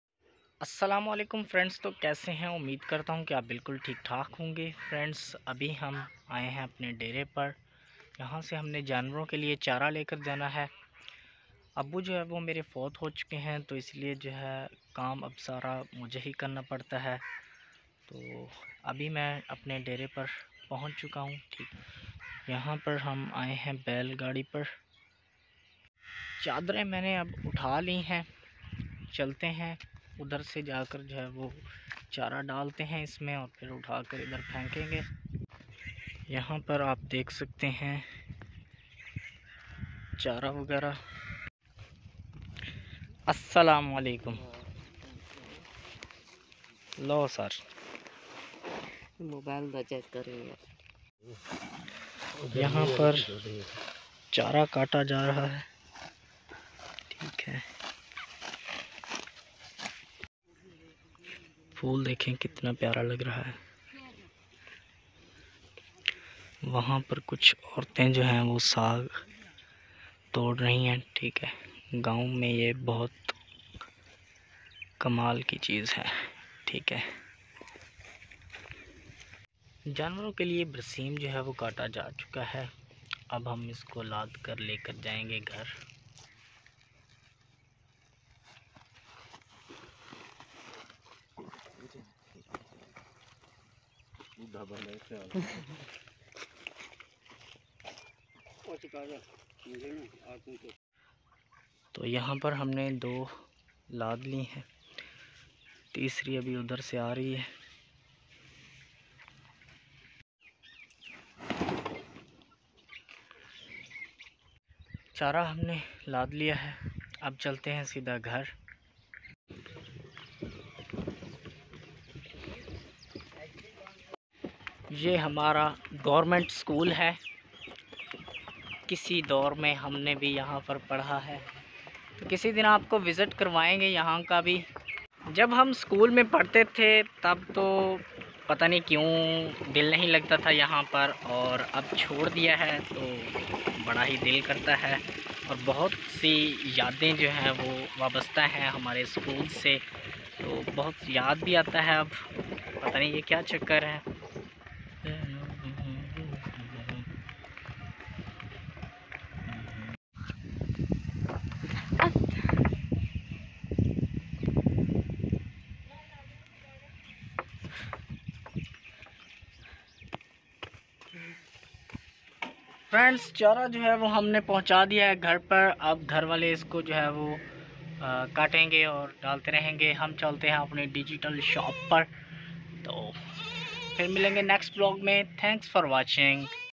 Aaj main charra ly k aaya | Janwaron ka Chara Gaaon sei | Grass for Our Animal | Village Vlog
Aaj main charra ly k aaya
Janwaron ka Chara Gaaon sei
Grass for Our Animal village Vlog